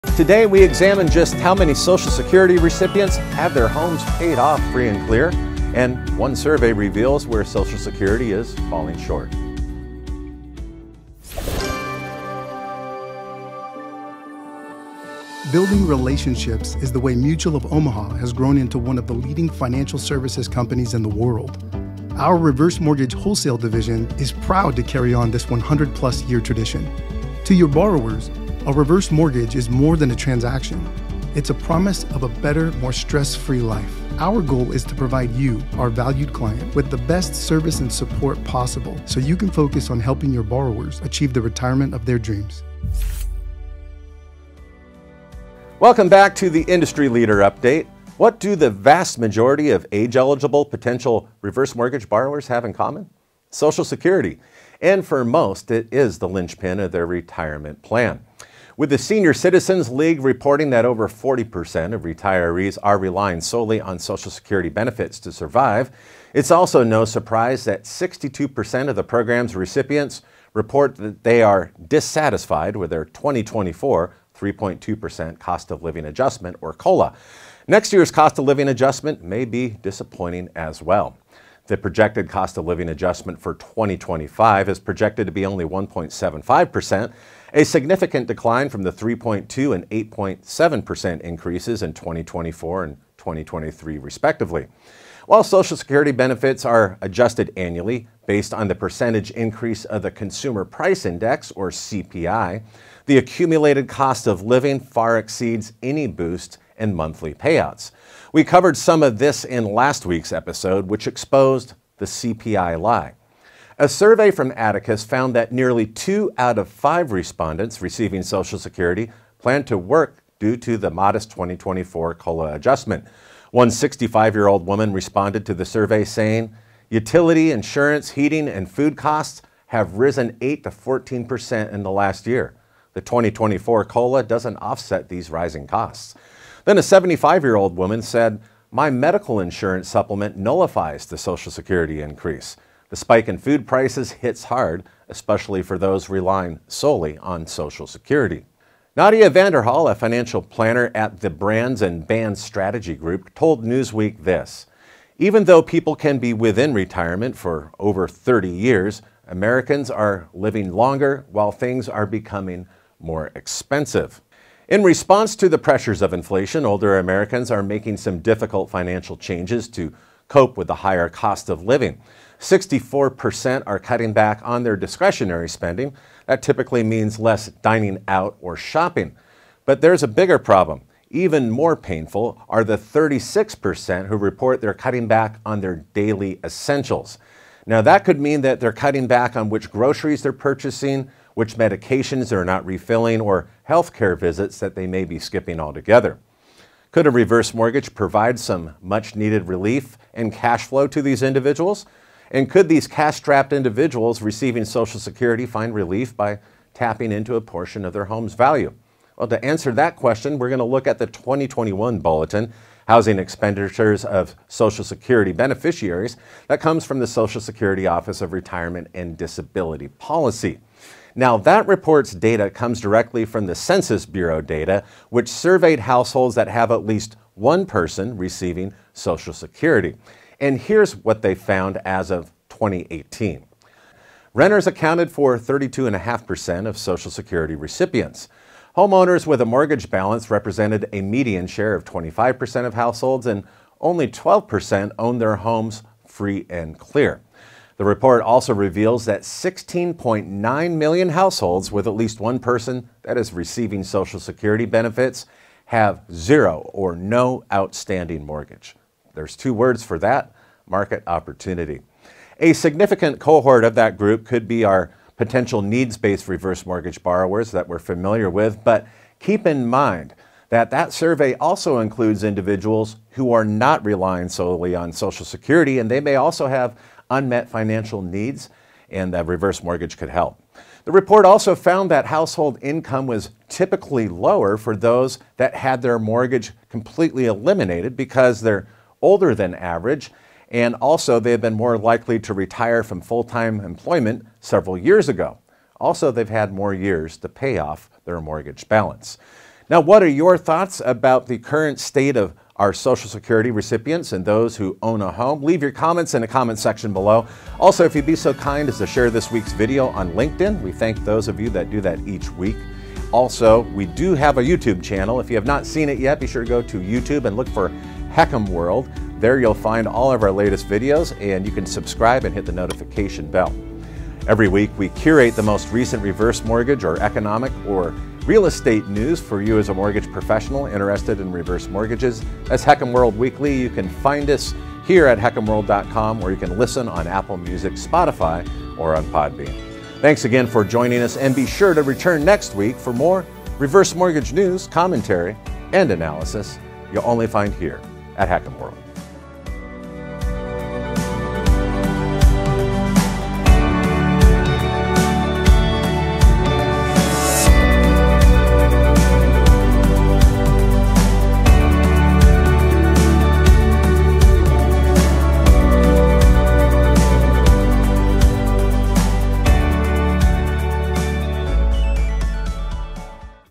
How many Social Security Recipients Have No Mortgage? The Industry Leader Update
What do the vast majority of age-eligible potential homeowners have in common? Social Security and for most it’s the linchpin of their retirement security.

With The Senior Citizens League reporting over 40 percent of retirees rely solely on Social Security benefits to survive, it is no surprise that 62% of program recipients report they are dissatisfied with their 2024 3.2% cost-of-living adjustment. Next year’s cost-of-living-adjustment may be disappointing as well. The projected cost-of-living adjustment for 2025 will be only 1.75 percent, a significant decline from the 3.2 and 8.7 percent increase in 2024 and 2023.

While Social Security benefits are adjusted annually based on the percentage increase of the Consumer Price Index (CPI) the accumulated cost of living far exceeds any boost in monthly payouts. We covered some of this in last week’s episode which exposed the CPI lie.

A survey from Atticus found nearly two out of five respondents plan to return to work due to the modest 2024 COLA increase. One 65-year-old woman responded to the survey saying, “Utility, insurance, heating, and food costs have risen 8-14% in the last year. The 2024 COLA doesn’t offset these rising costs”. A 75-year-old woman said, “My medical insurance supplement nullifies the Social Security increase. The spike in food prices hits hard, especially for those relying solely on Social Security.”

Nadia Vanderhall, a financial planner at The Brands and Bands Strategy Group, told Newsweek, “Even though people can be within retirement for over 30 years, Americans are living longer while things are becoming more expensive."

In response to the pressures of inflation, older Americans are making financial changes to cope with the higher cost of living. 64% are cutting back on their discretionary spending. This typically means less dining out or shopping. However, even more painful are the 36% who are cutting back on daily essentials. Consequently, older Americans are cutting back on groceries, medications, or healthcare visits.

Could a reverse mortgage provide some much-needed ca  sh flow? Could these cash-strapped Social Security beneficiaries find relief by tapping into their home’s value? To answer that question we look at the 2021 bulletin Housing Expenditures of Social Security Beneficiaries from the Social Security Office of Retirement and Disability Policy. The report data comes from Census Bureau data that surveyed households with at least one person receiving Social Security. Here’s what they found as of 2018. Renters accounted for 32.5% of Social Security recipients. Homeowners with a mortgage balance represented a median share of 25% of households, and only 12% owned their homes free and clear.

The report also reveals that 16.9 million households with at least one person receiving Social Security benefits have no outstanding mortgage. A significant cohort of this group could be potential needs-based reverse mortgage borrowers. However, we must be mindful that the survey includes those who are not relying solely on Social Security and who also have unmet financial needs. The report also found that household income was typically lower for homeowners without a mortgage being older than average explaining this group has been more likely to retire from full-time employment several years ago and have more years to pay off their mortgage balance.